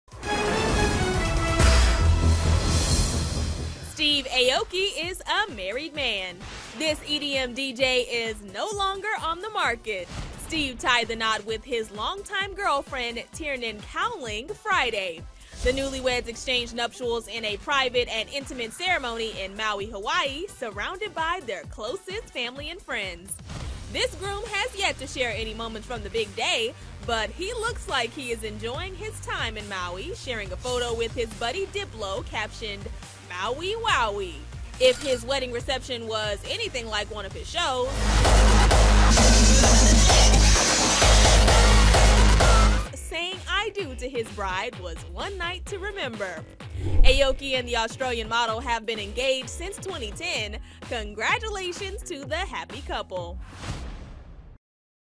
DJ Steve Aoki Ties The Knot With Longtime Girlfriend Tiernan Cowling
The EDM DJ is a married man!